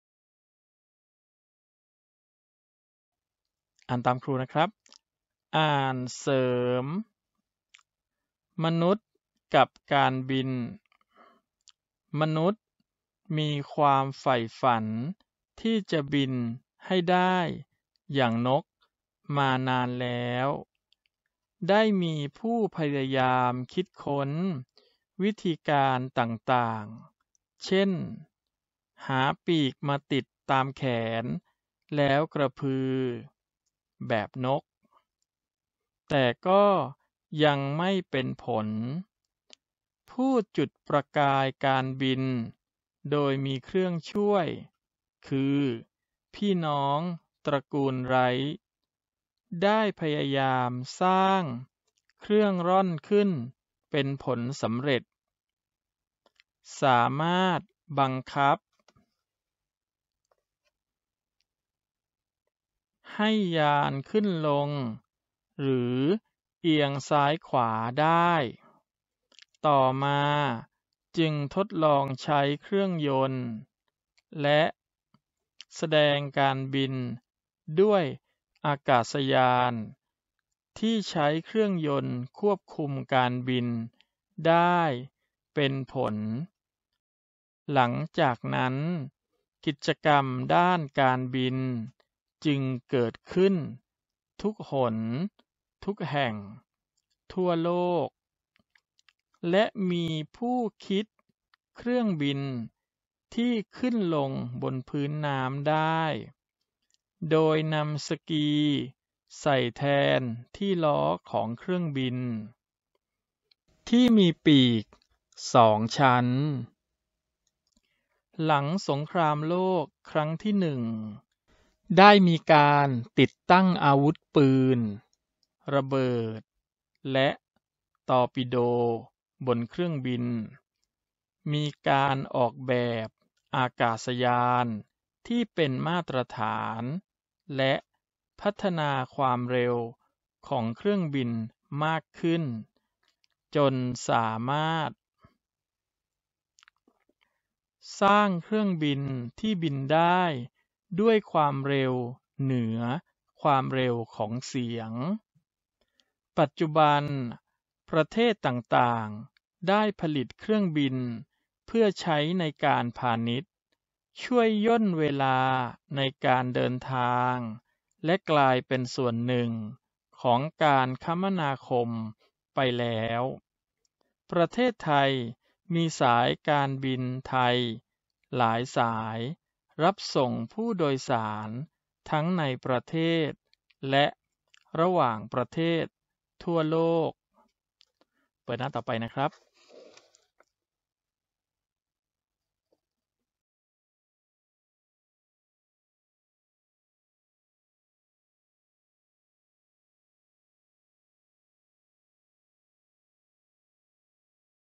ภาษาพาที ป. 3 บทที่ 7 เรื่อง ความฝันเป็นจริงได้ ตอนที่ 3
ฝึกทักษะการอ่านวิชาภาษาไทยสำหรับเด็กชั้นประถมศึกษาปีที่ 3 ชุดภาษาพาที บทที่ 7 เรื่อง "ความฝันเป็นจริงได้"

ถือเป็นกำลังใจในการผลิตออกมาอีก ขอบคุณครับ

หนังสือที่ใช้ประกอบกับคลิปนี้คือหนังสือรายวิชาพื้นฐานภาษาไทย ชุด ภาษาเพื่อชีวิต ภาษาพาที ชั้นประถมศึกษาปีที่ 3 กลุ่มสาระการเรียนรู้ภาษาไทย ตามหลักสูตรแกนกลางการศึกษาขึ้นพื้นฐาน พุทธศักราช 2551

คลิปแนะนำอาจารย์ผู้สอน